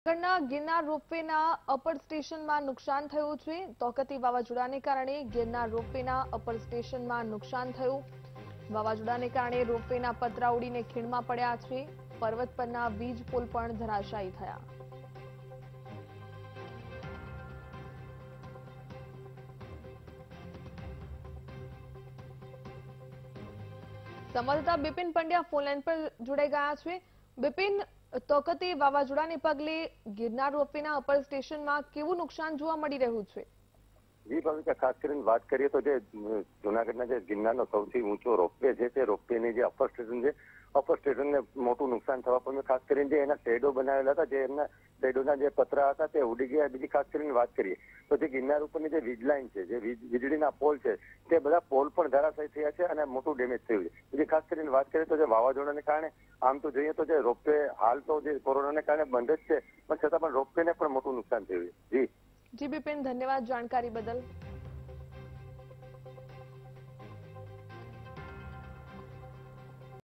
Tauktae Cyclone ના કારણે Junagadh ના ગિરનાર રોપ-વેના અપર સ્ટેશનમાં નુકસાન | VTV Gujarati
Tauktae Cyclone ના કારણે Junagadh ના ગિરનાર રોપ-વેના અપર સ્ટેશનમાં નુકસાન